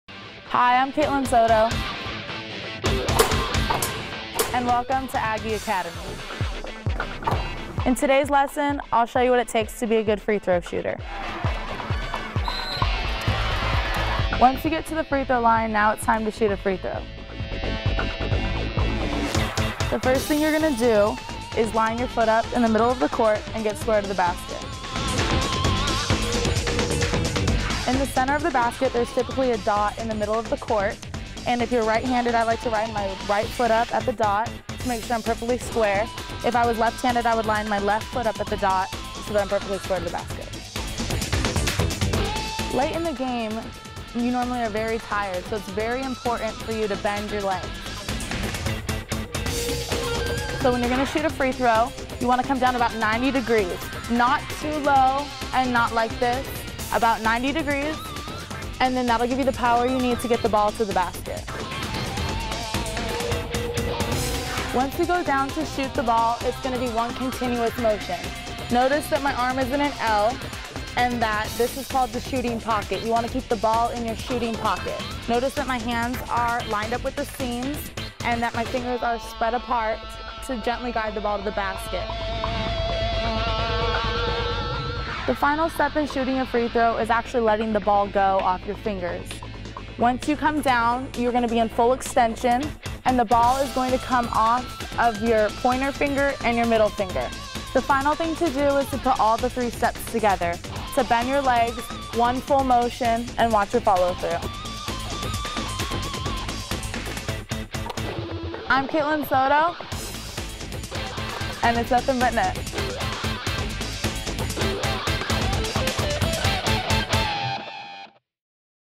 Aggie Academy - Free Throw Shooting
Sophomore guard Kaitlyn Soto shows the steps to a perfect free-throw attempt